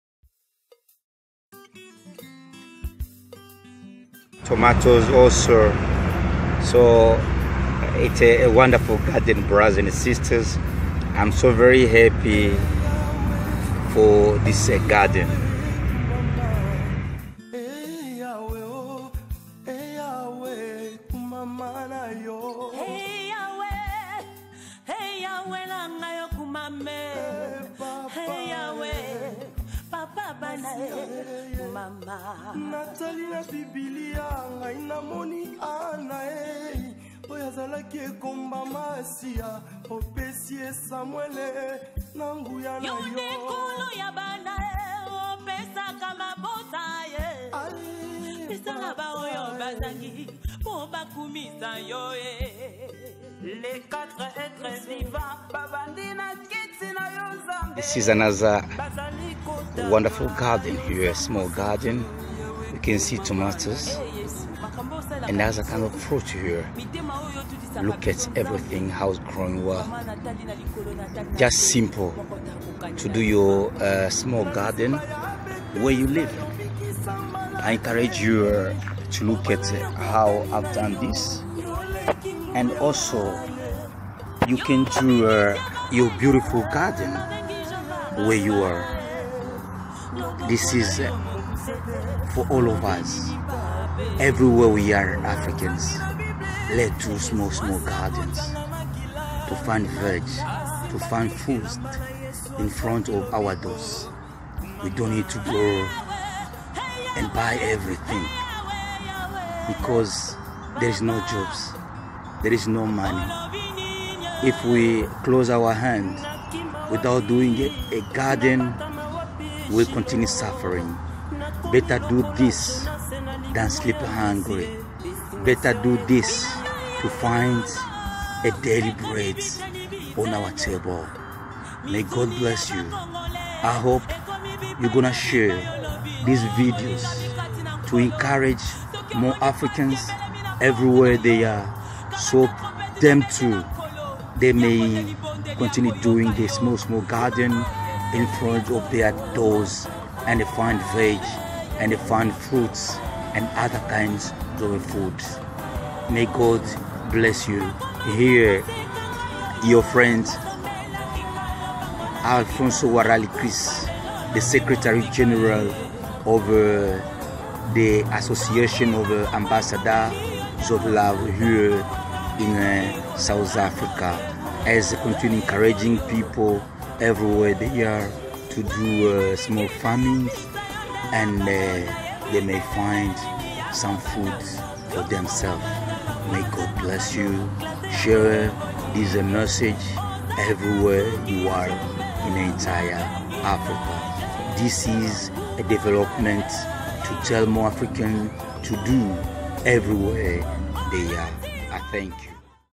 HELP YOURSELVES BY DOING SMALL GARDENS/ BY AMBASSADORS OF LOVE
Can you do your garden in this hard time? It is good way to fight against hunger.